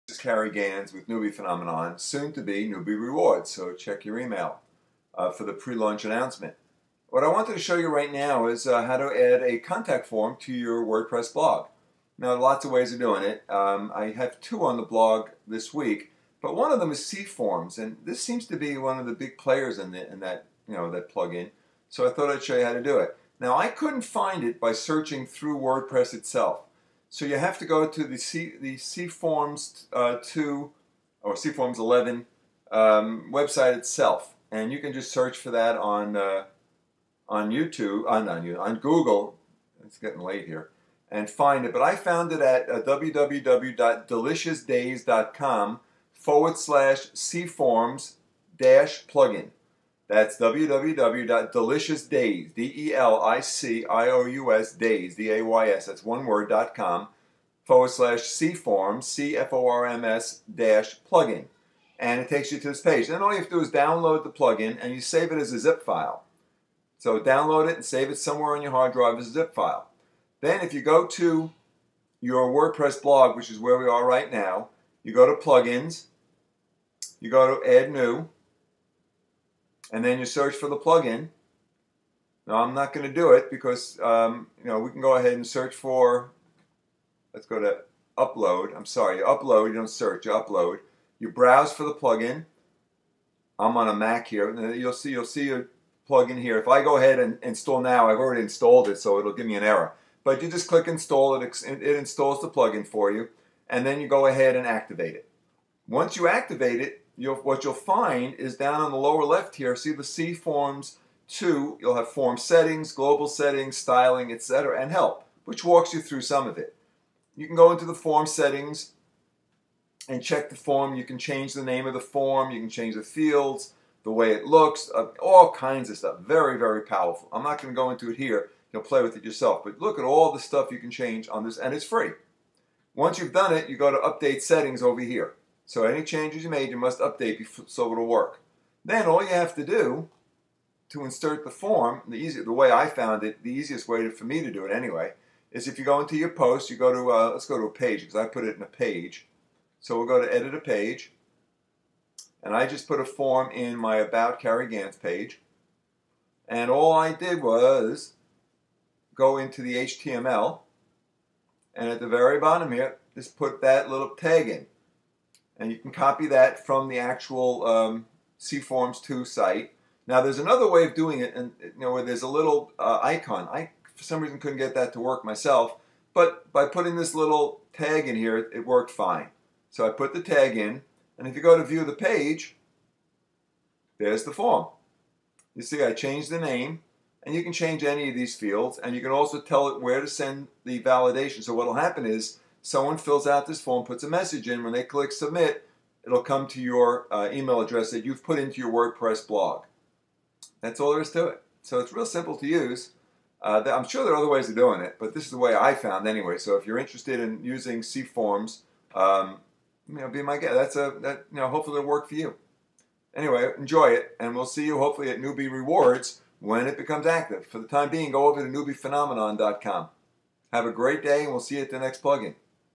WordPress Plug-in of the Day - Adding Contact Forms
If you're interested in adding contact forms to your WordPress blog you need to watch this video and then go to our blog at http:/www